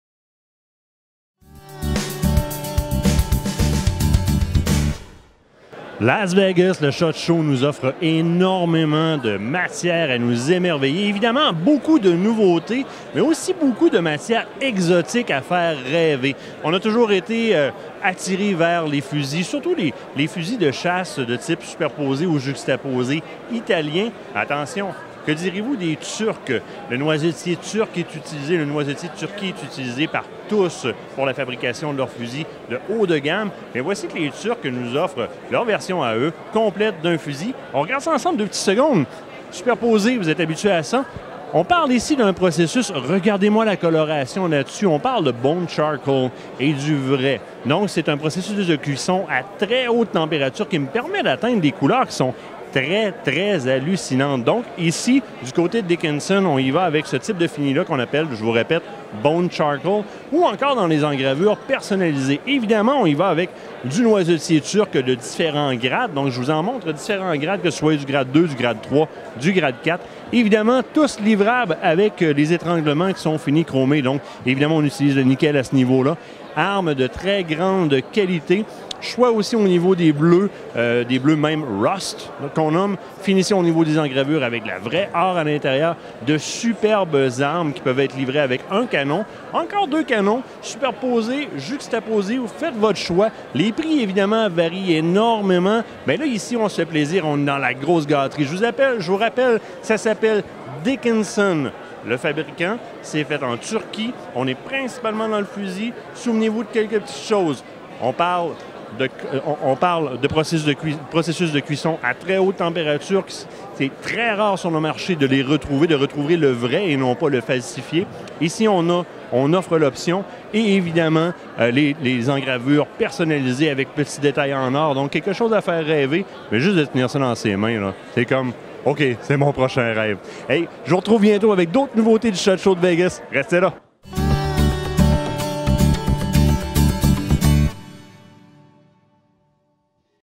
Dickinson Firearms - Shot How 2017 Las Vegas - Martin Bourget
Shot How 2017 Las Vegas - Martin Bourget -Dickinson Firearms

Martin Bourget est au Shot Show 2017 à Las Vegas dans le kiosque de Dickinson Firearms question de nous présenter cette gamme de fusils Turcs haut de game.